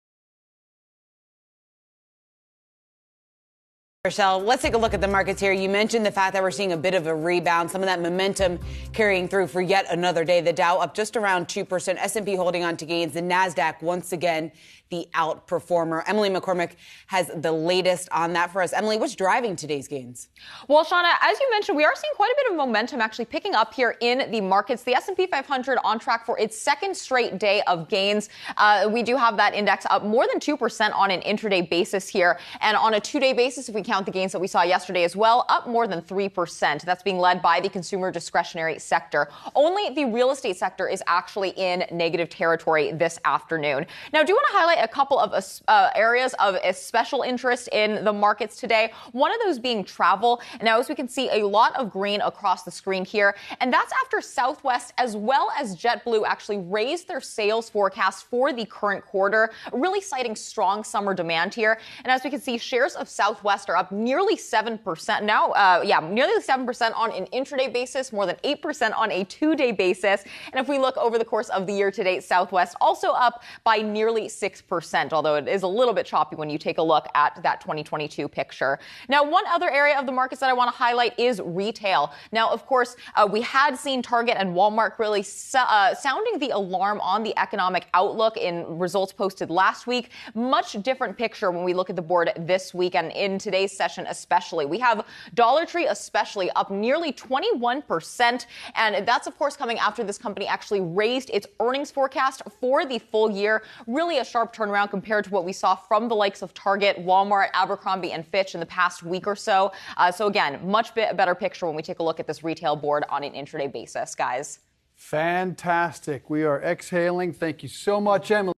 S&P 500 on track for best day in two weeks, travel stocks lead market rebound
Yahoo Finance's Emily McCormick breaks down how stocks are trading on Thursday afternoon.